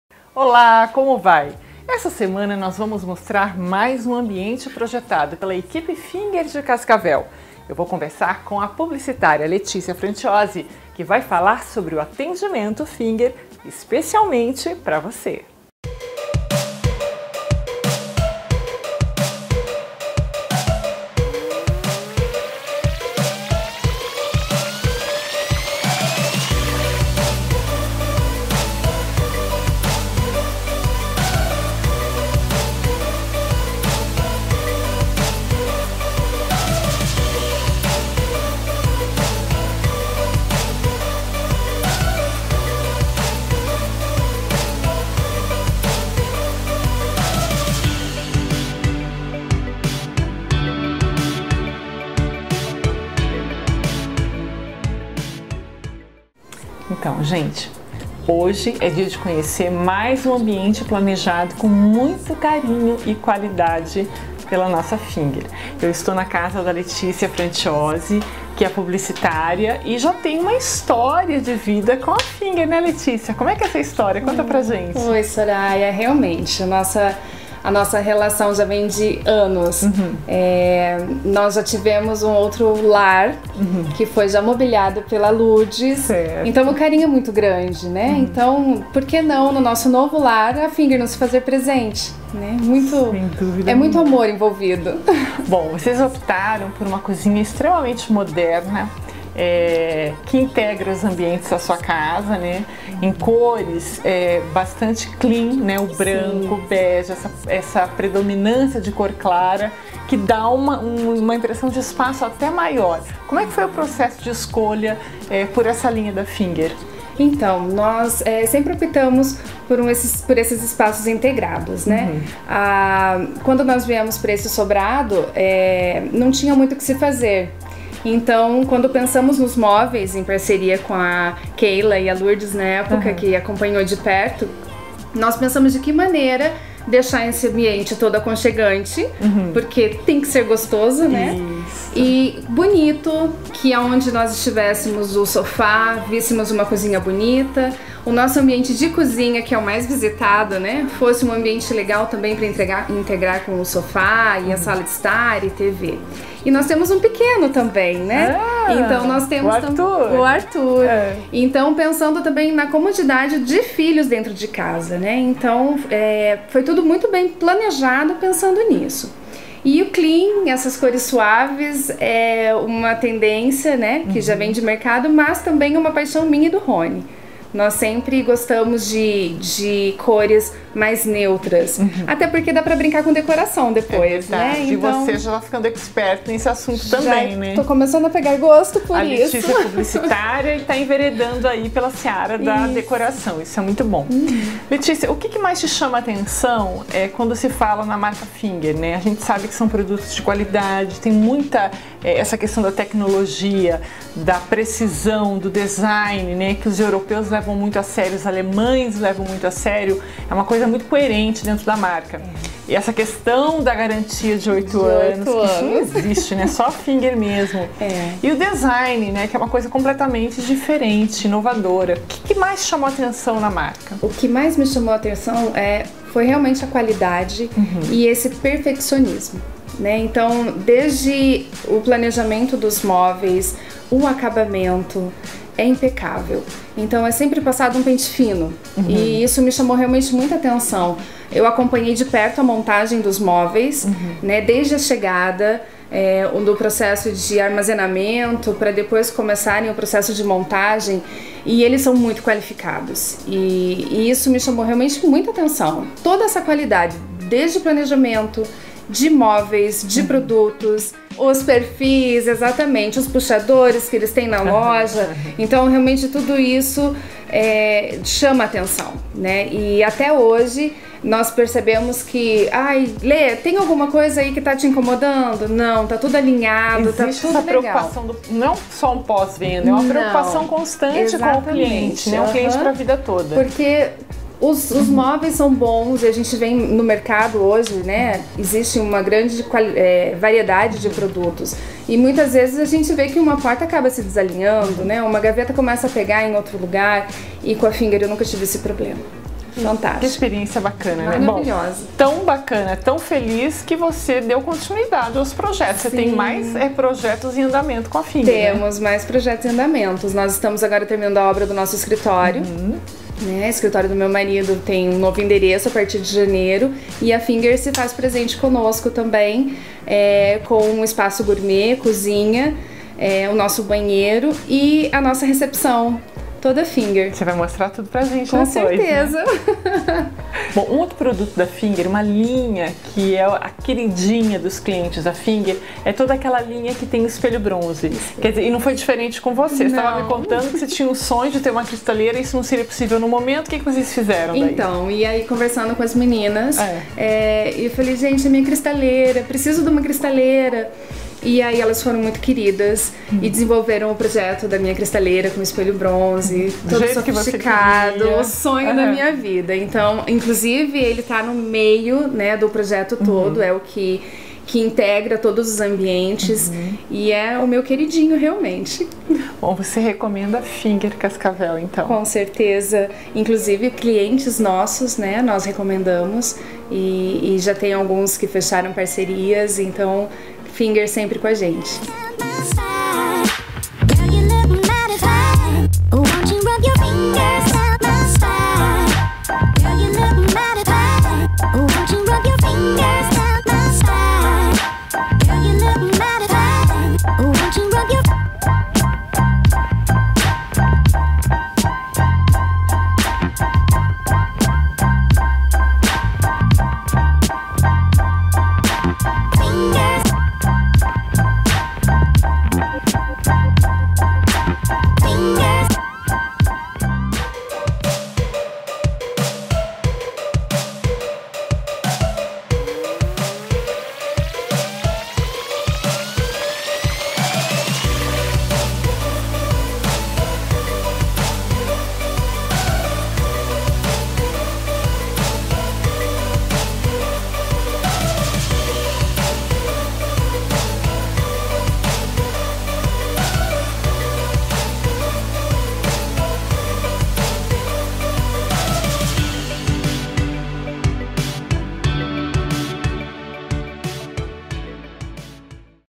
FINGER & VOCÊ - Atendimento Finger Cascavel
Neste programa você conhece mais um projeto da Finger Cascavel, executado com muito carinho. Conheça a experiência da publicitária Letícia Frantiozi com a Finger!